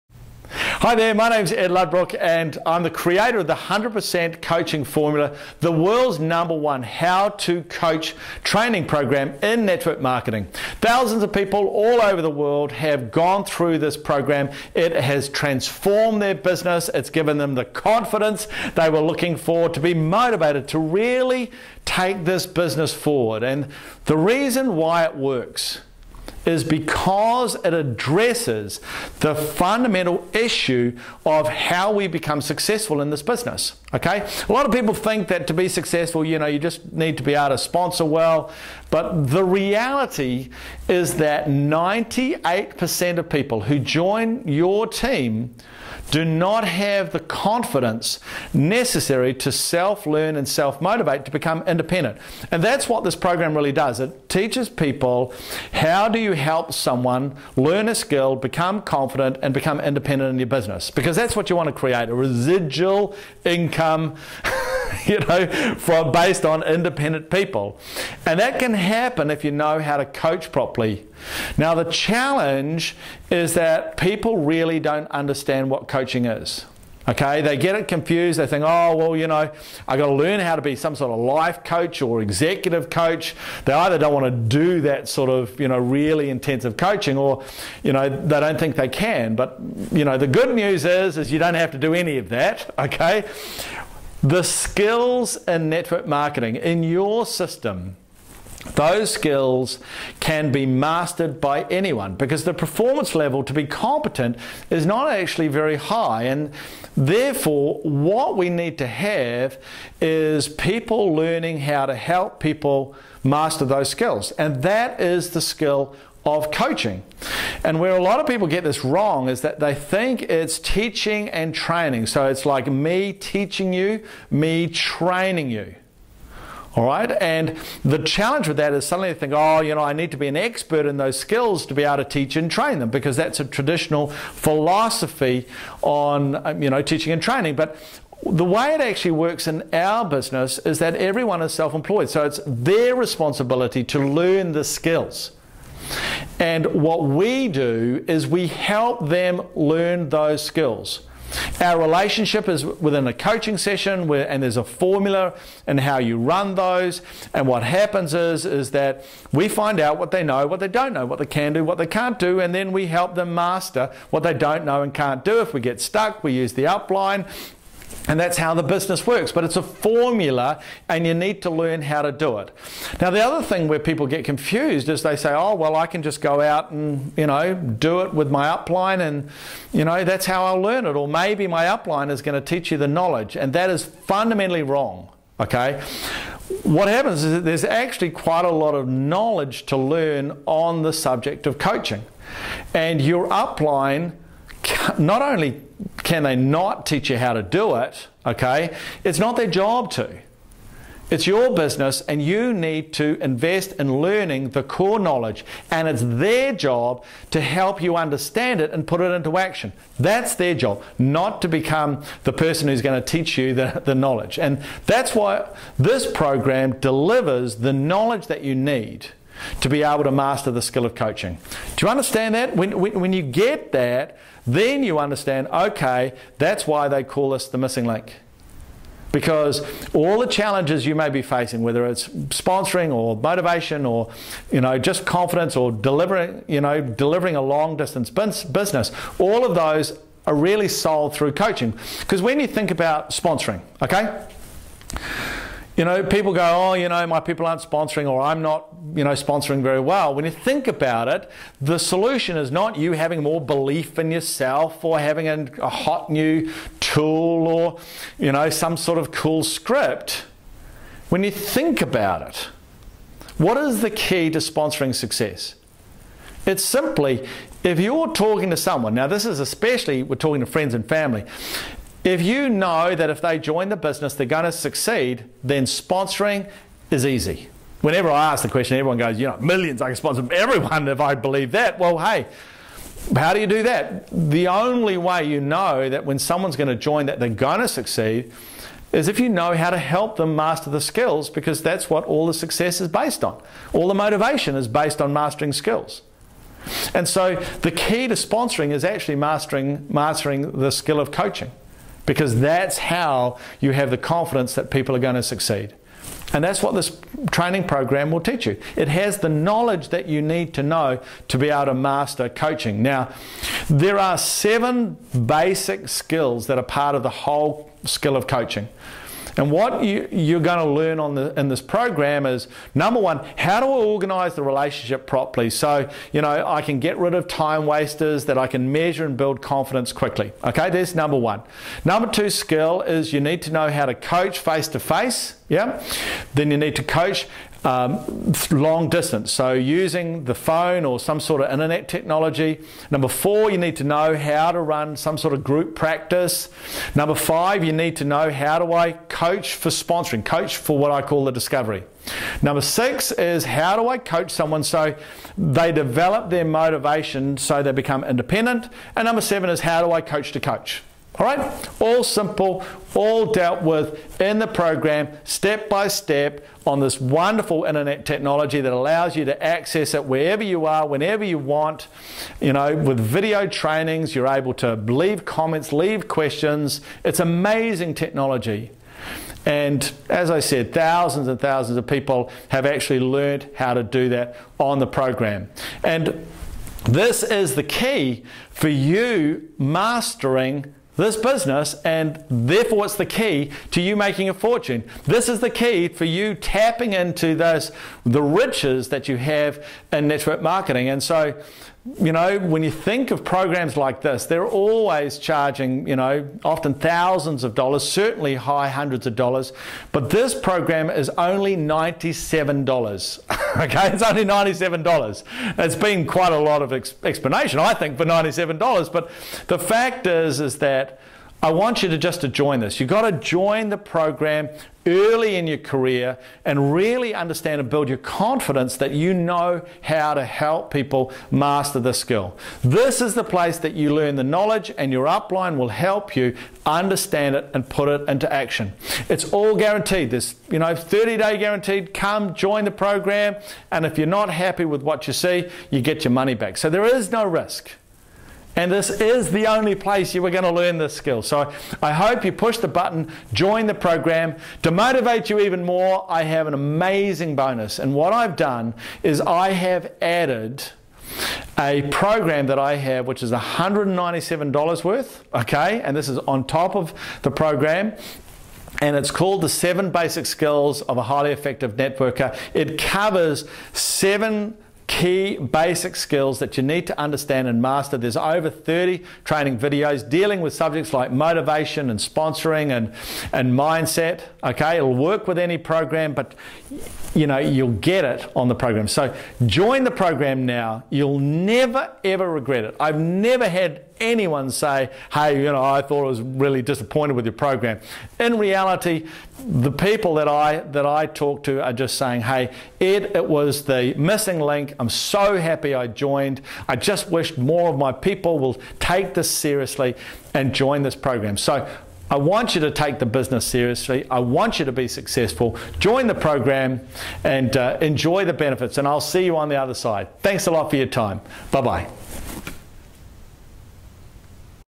Join the 100% Coaching Formula
Ed explains why you must join the 100% coaching program today!